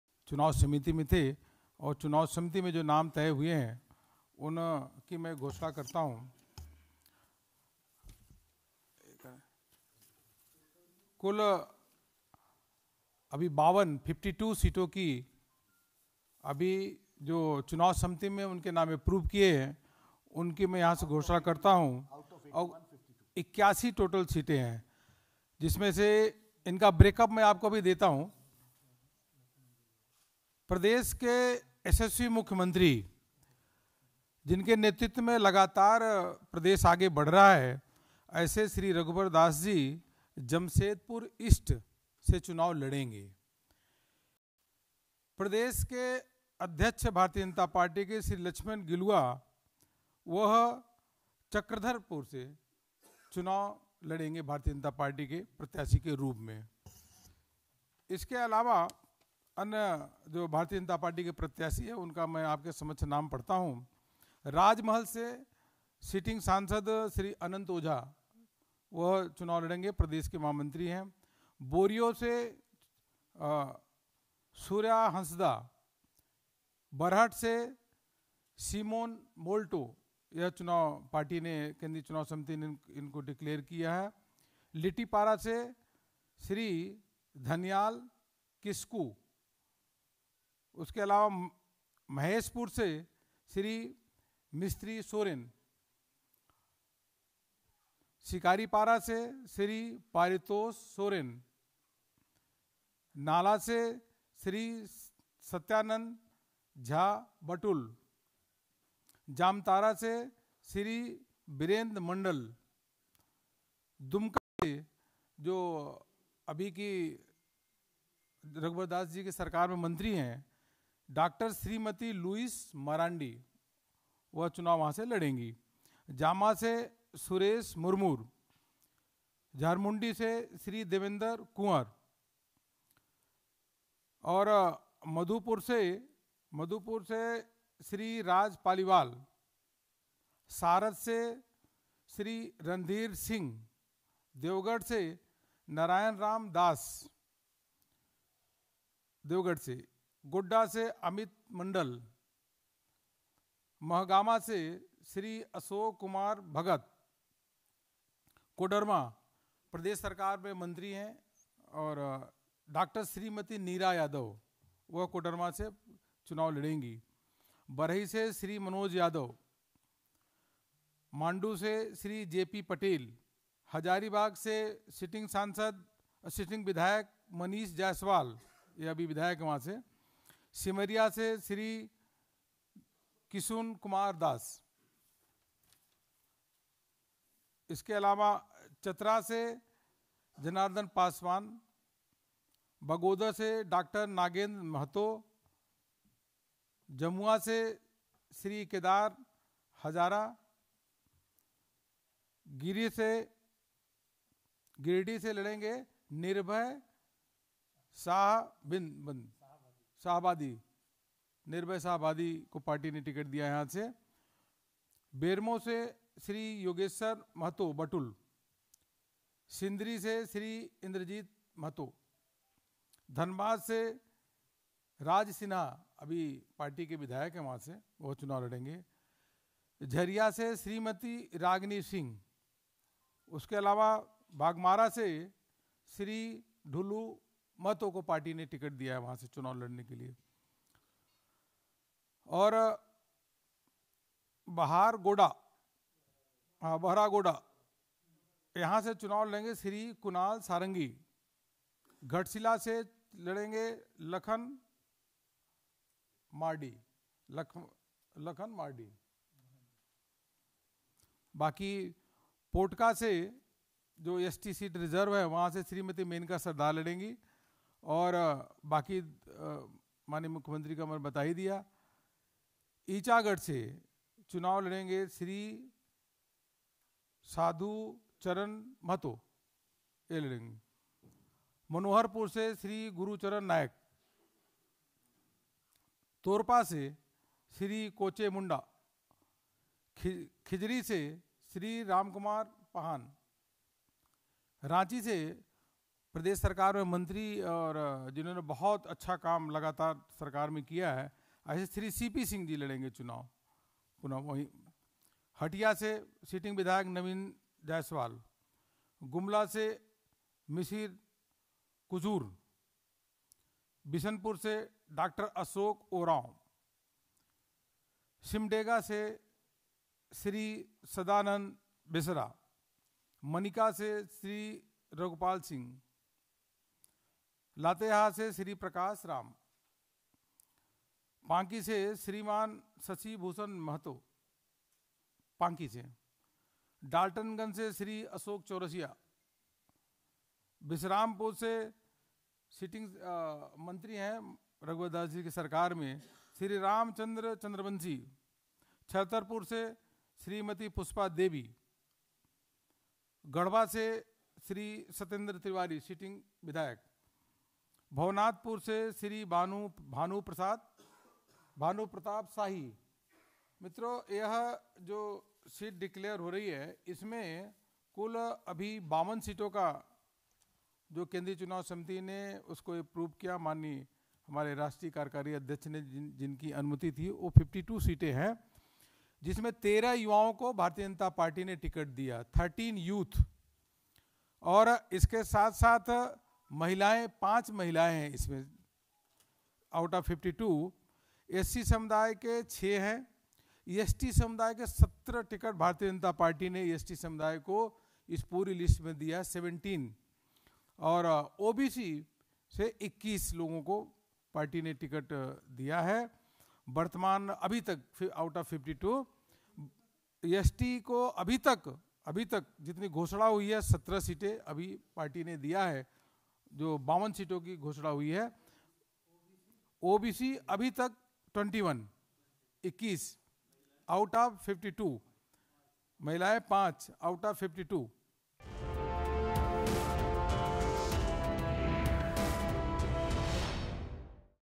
झारखंड विधानसभा चुनाव: बीजेपी की पहली लिस्ट जारी, किसका कटा पत्ता?
झारखंड विधानसभा चुनाव के लिए बीजेपी की पहली लिस्ट जारी, किसका कटा पत्ता?